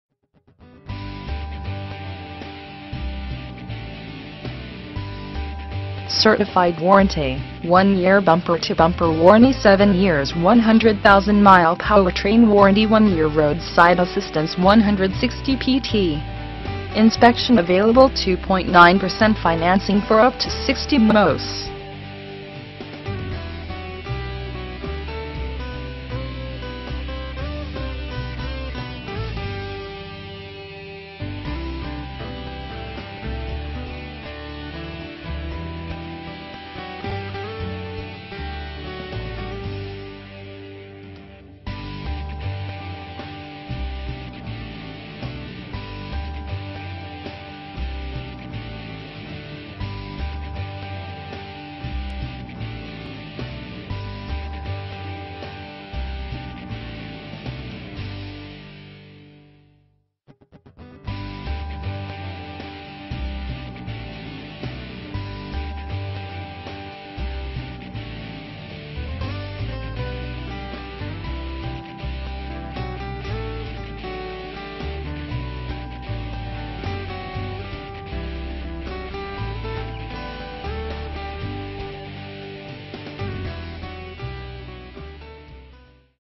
2009 Toyota Corolla - Classic Dealer Group
For Sale: 2009 Toyota Corolla
VIN: 1NXBU40E19Z047118
Engine: Gas I4 1.8L/110
Drivetrain: FWD
Transmission: Automatic
Mileage: 50,083
Color: Magnetic Gray Metallic (exterior) Ash (interior)